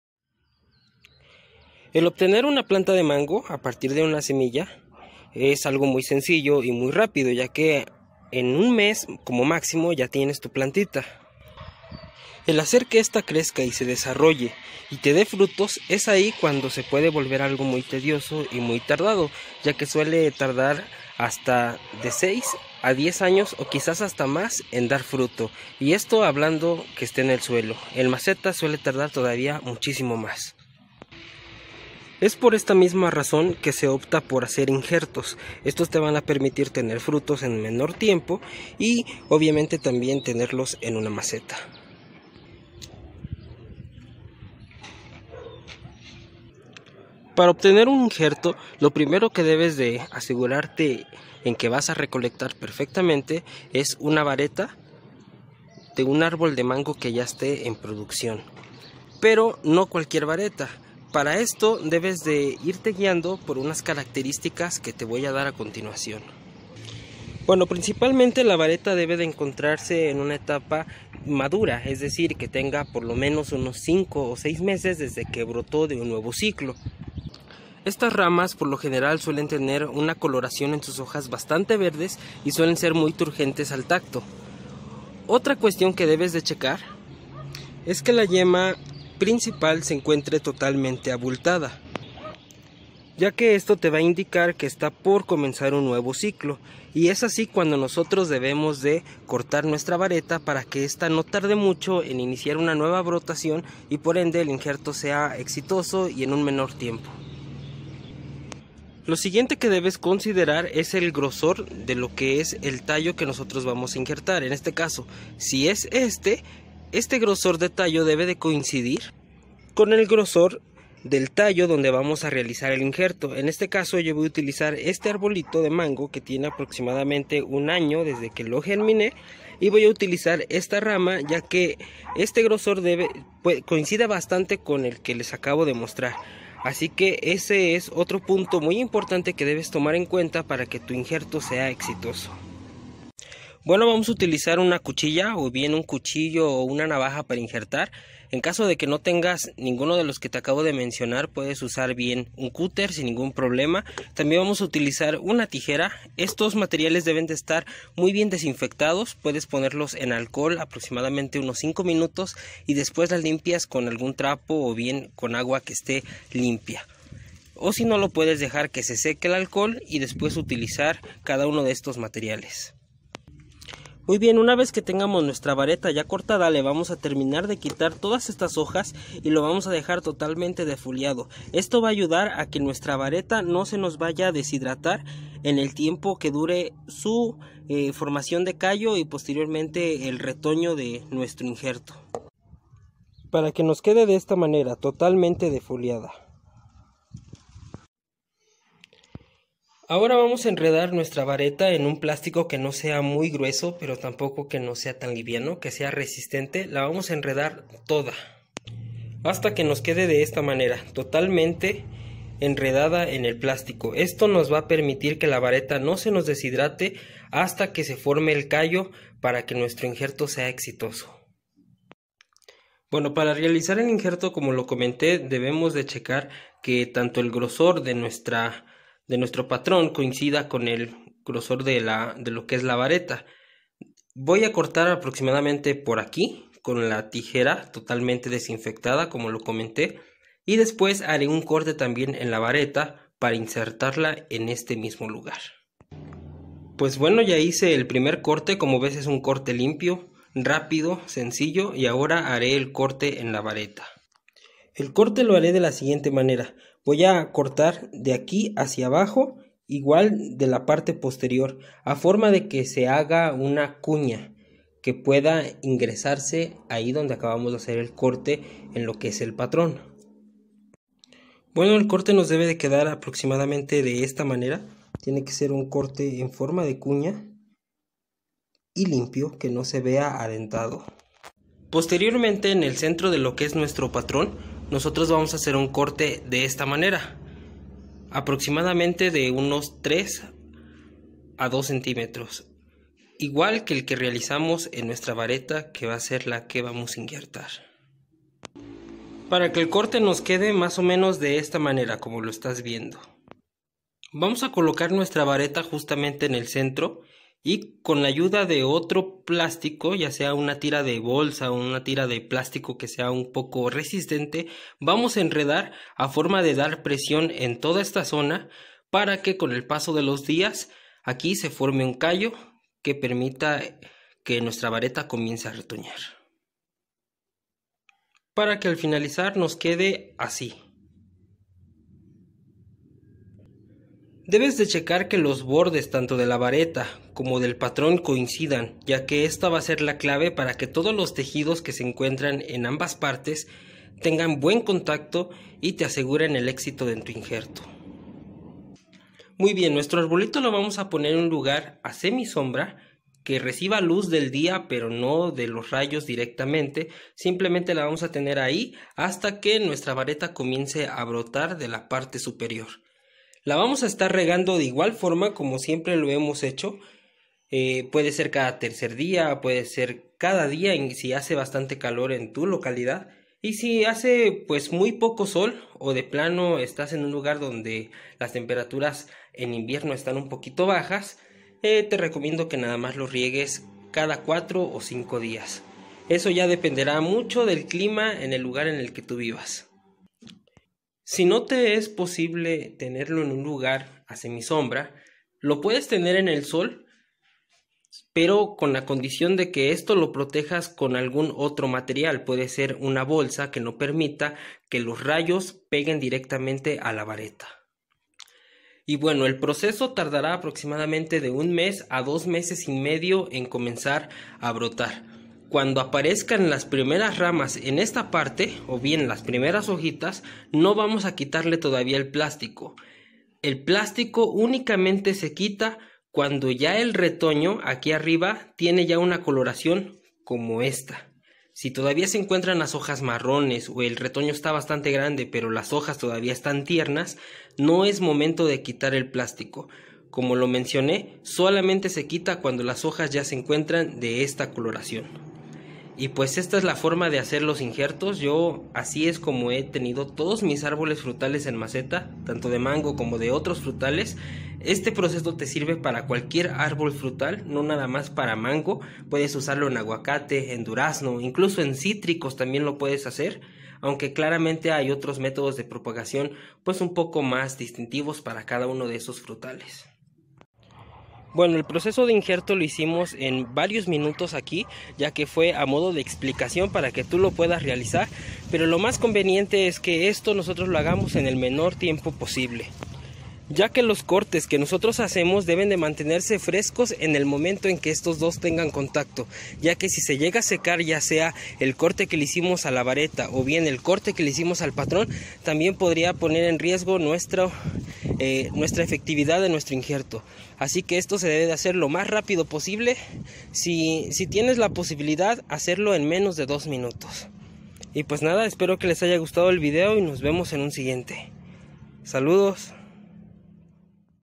Como hacer un INJERTO - Paso a paso - hvordan pode mango - Graft on mango - how to make a graft
en este video muestro como hacer un injerto, desde la descripción del mismo, la elección de la vareta y el proceso de principio a fin, espero que sea de su agrado el video.
SALUDOS.

00:00 Introducción
00:47 Recolección de la vareta
02:24 Materiales a usar
02:53 Preparación de la vareta
04:06 Inicio del injerto
05:48 Cuidados post injerto

MHT 2022.